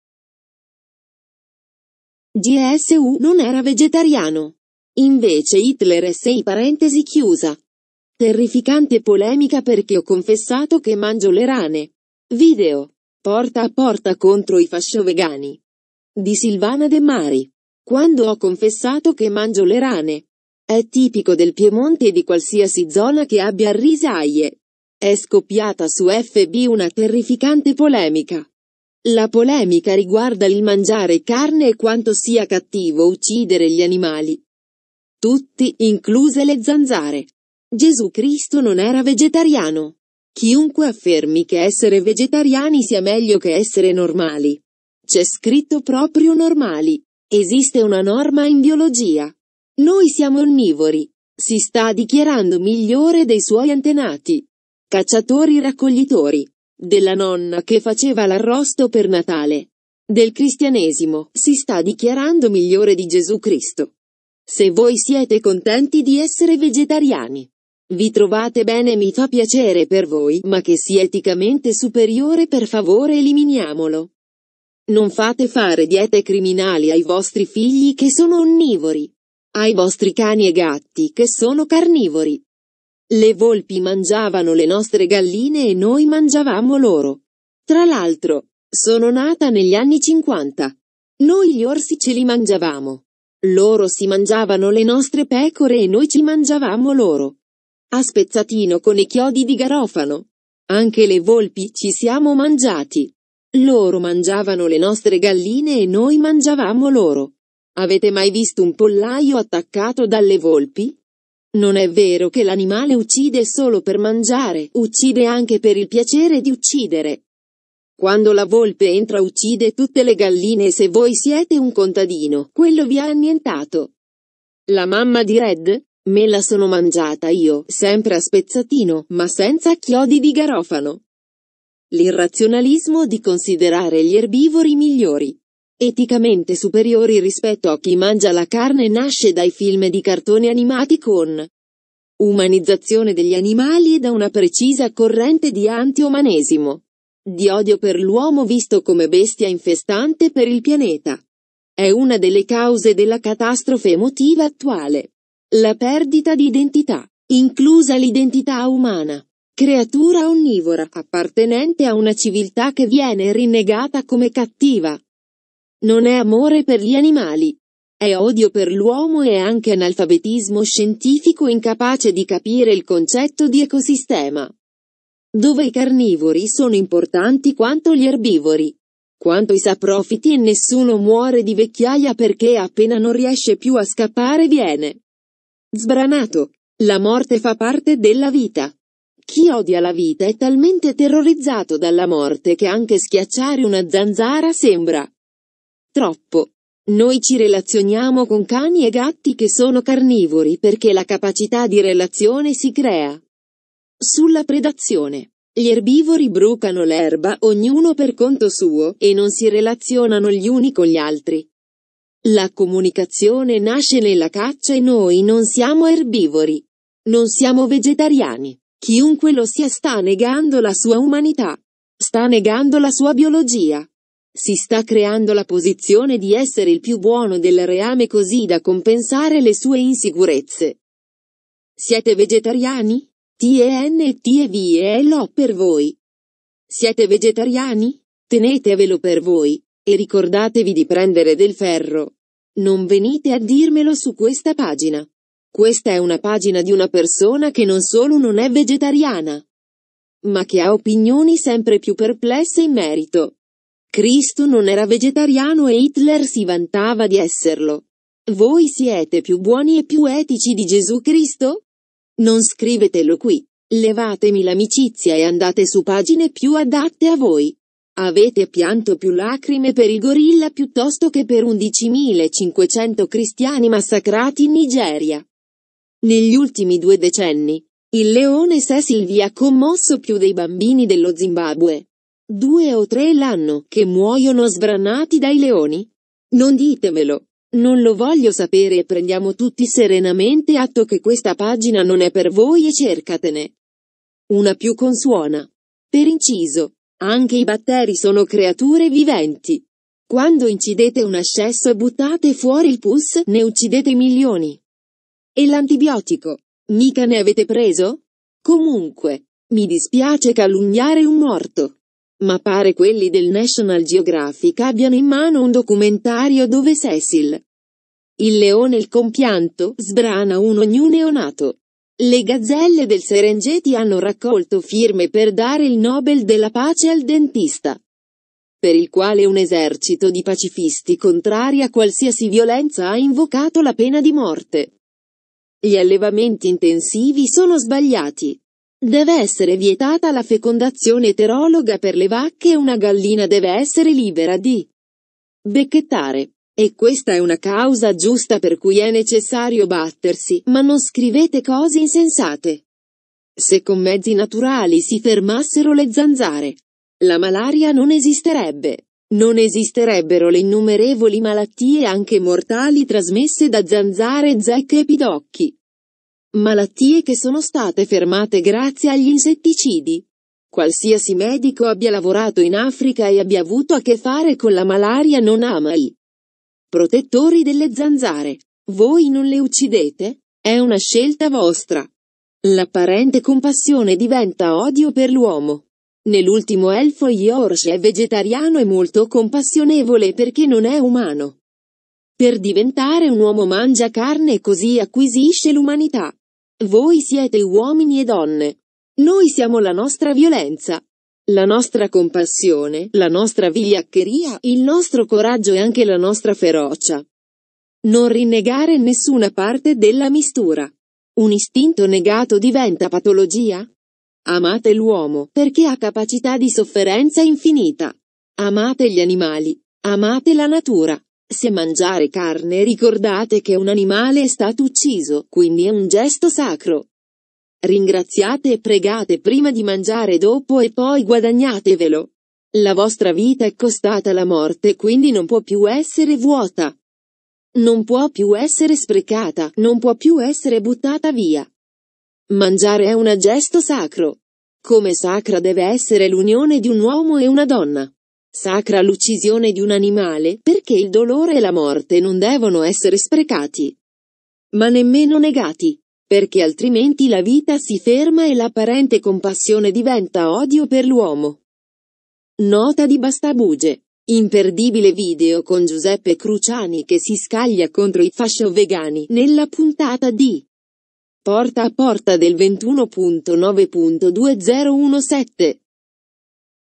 GESU' NON ERA VEGETARIANO (INVECE HITLER SI) di Silvana De Mari
Terrificante polemica perché ho confessato che mangio le rane
BastaBugie n.542 del 17 gennaio 2018